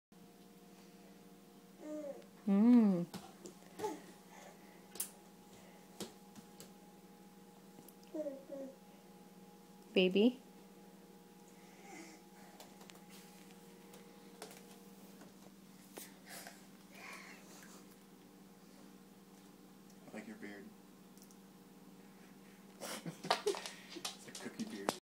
She likes Oreos
Madeleine with a cookie beard.